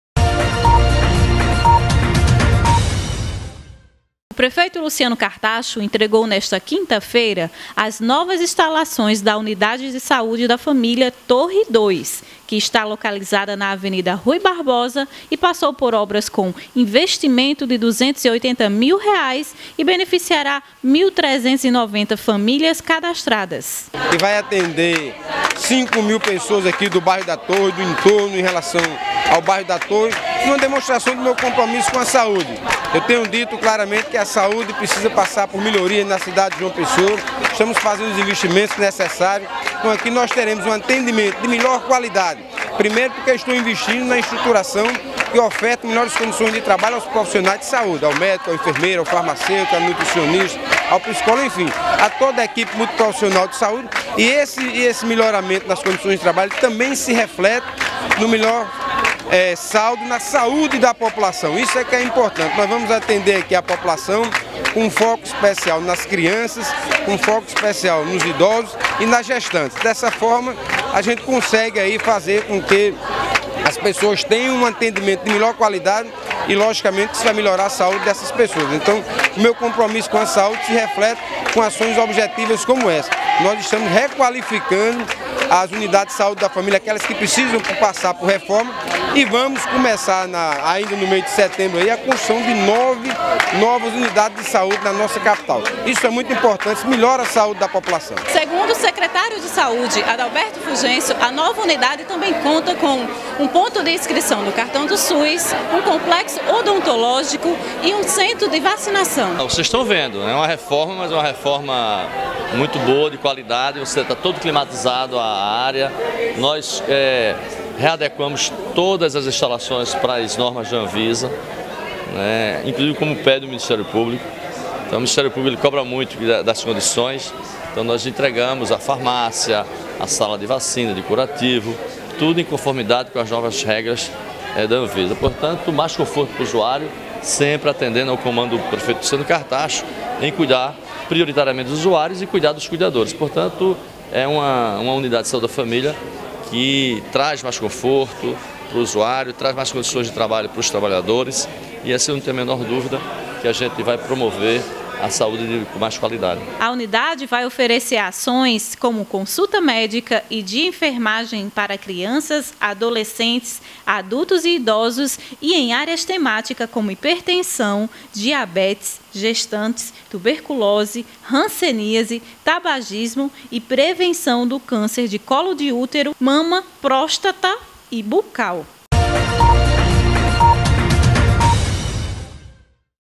Nova Unidade de Saúde da Família Torre II beneficiará 1390 familias
O prefeito Luciano Cartaxo entregou nesta quinta-feira (22), as novas instalações da Unidade de Saúde da Família (USF) Torre II, localizada na Avenida Rui Barbosa, que passou por obras com investimento de R$ 280 mil e beneficiará 1.390 famílias cadastradas - ou cerca de cinco mil pessoas.

"A entrega dessa USF representa o nosso compromisso com a melhoria das condições de trabalho dos profissionais da saúde e principalmente, de garantir mais qualidade no serviço prestado a população. Estamos trabalhando para melhorar a saúde do nosso povo", afirmou o prefeito.